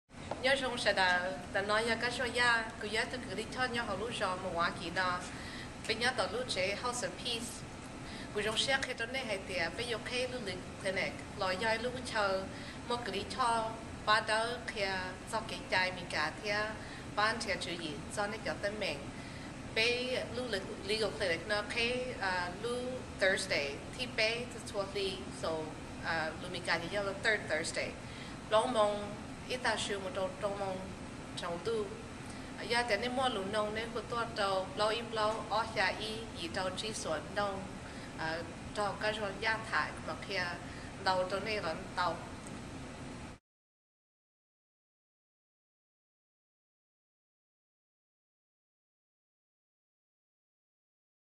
The House of Peace offers free legal advice on civil matters to Hmong-speaking residents. (Hmong)
-Capuchin social service ministry, serving Milwaukee's poor with food pantry, clothing closet, Stritch teen program, UWM Nursing and Marquette Legal Clinics